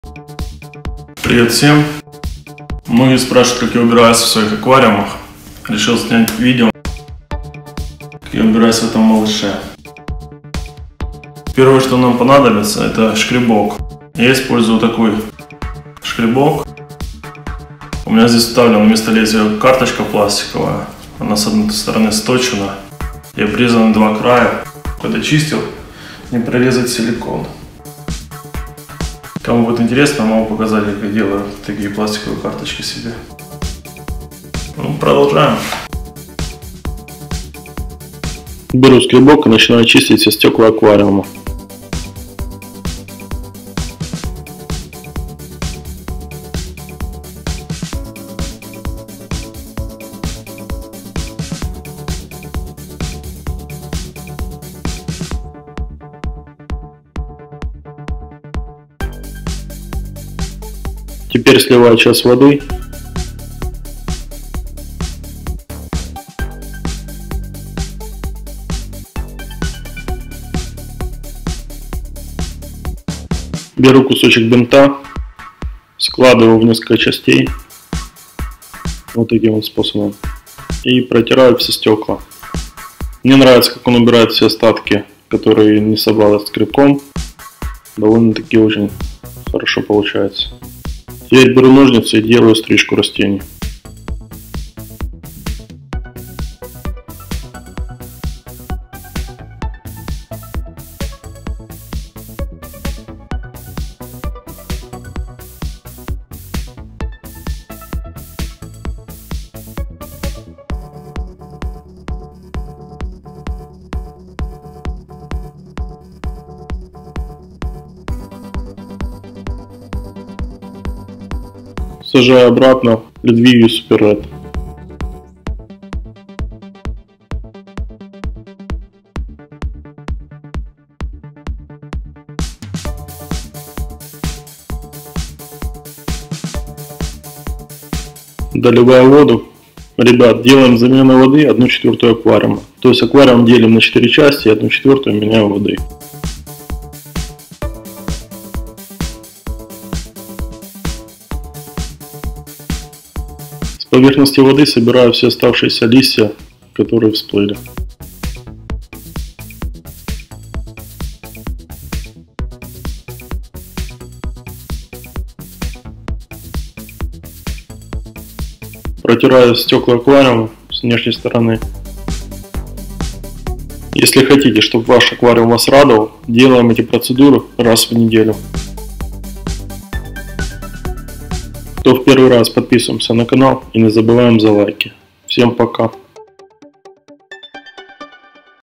Как я убираю в своем 72 литровом аквариуме!!!
1)Аквариум 180 лита
Размер 90х45х45 см
Освещение (на канале)
Фильтр JBL CristalProfi e1501
Грунт Aquayer Питательная подложка, JBL Manado
Вода осмос + соли GH8 KH 0-3
CO2 да + удобрения

2)Аквариум 72 лита
Размер 60х30х40 см
Освещение (на канале)
Фильтр SUNSUN HW-603B
Грунт Fluval PLANT & SHRIMP
Вода осмос + соли GH7 KH 0-1
CO2 да + удобрения